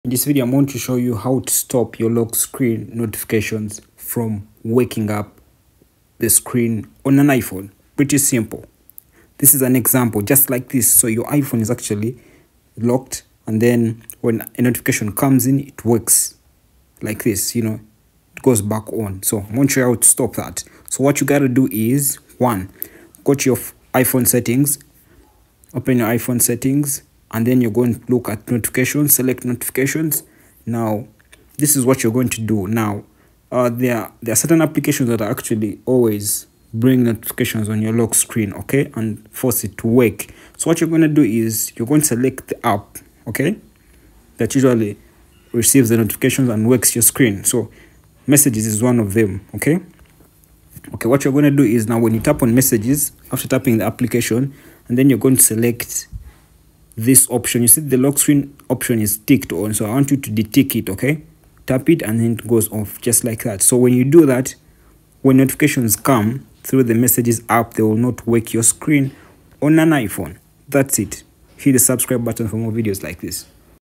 How To Stop Lock Screen Notifications From Waking up Screen iPhone 2024
In this video, I am going to show you how to stop Lock screen Notifications From Waking up Screen which is locked on an iPhone. iOS 17 on your device. including  stopping app Notifications From Waking up a Lock screen on iphone